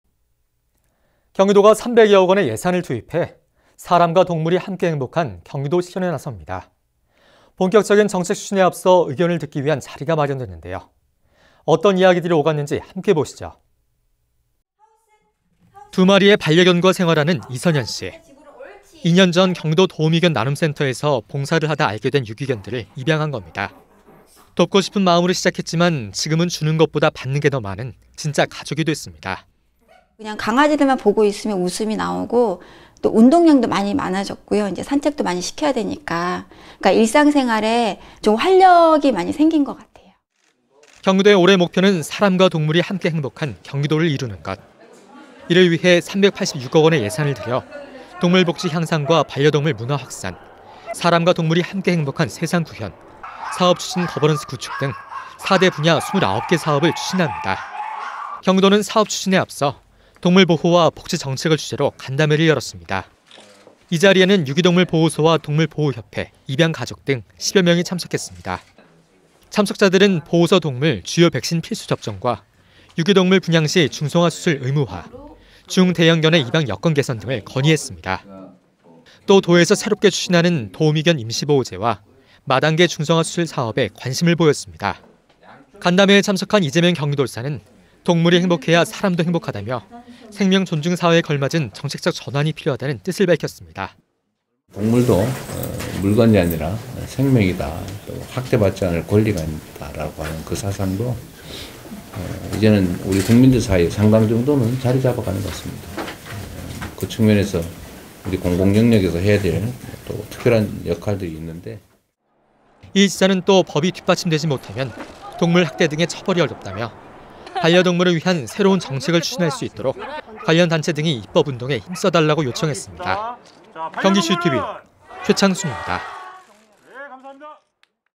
이재명 지사 “생명존중사회 걸맞은 정책 전환 필요”
[앵커멘트] 경기도가 3백여 억 원의 예산을 투입해 사람과 동물이 함께 행복한 경기도 실현에 나섭니다. 본격적인 정책 추진에 앞서 의견을 듣기 위한 자리가 마련됐는데요. 어떤 이야기들이 오갔는지 최창순 기자가 보도합니다.

[리포트]
두 마리의 반려견과 생활하는 이선연 씨

2년 전 경기도도우미견나눔센터에서 봉사를 하다 알게 된 유기견들을 입양한 겁니다.

돕고 싶은 마음으로 시작했지만 지금은 주는 것보다 받는 게 더 많은 진짜 가족이 됐습니다.

[인터뷰] 이선연 / 유기견 입양가족
“그냥 강아지들만 보고 있으면 웃음이 나오고 또 운동량이 많이 많아졌고요. 산책도 많이 시켜야 되니까 일생생활에 좀 활력이 많이 생긴 것 같아요.”

경기도의 올해 목표는 사람과 동물이 함께 행복한 경기도를 이루는 것

이를 위해 386억 원의 예산을 들여 동물복지 향상과 반려동물 문화 확산, 사람과 동물이 함께 행복한 세상 구현, 사업 추진 거버넌스 구축 등 4대 분야 29개 사업을 추진합니다.

경기도는 사업 추진에 앞서 동물보호와 복지 정책을 주제로 간담회를 열었습니다.

이 자리에는 유기동물보호소와 동물보호협회, 입양가족 등 10여 명이 참석했습니다.

참석자들은 보호소 동물 주요 백신 필수 접종과 유기동물 분양 시 중성화 수술 의무화, 중·대형견의 입양 여건 개선 등을 건의했습니다.

또 도에서 새롭게 추진하는 도우미견 임시보호제와 마당 개 중성화 수술 사업에 관심을 보였습니다.

간담회에 참석한 이재명 경기도지사는 동물이 행복해야 사람도 행복하다며 생명존중사회에 걸맞은 정책적 전환이 필요하다는 뜻을 밝혔습니다.

“동물도 물건이 아니라 생명이다 또 학대받지 않을 권리가 있다고 하는 사상도 이제는 국민들 사이에 상당 정도는 자리 잡은 것 같습니다. 그 측면에서 공공영역에서 해야 될 특별한 역할들이 있는데요.”

이 지사는 또 법이 뒷받침되지 못하면 동물학대 등의 처벌이 어렵다며, 반려동물을 위한 새로운 정책을 추진할 수 있도록 관련 단체 등이 입법 운동에 힘써달라고 요청했습니다.

영상취재 : 김현우 나인선, 영상편집 : 김정환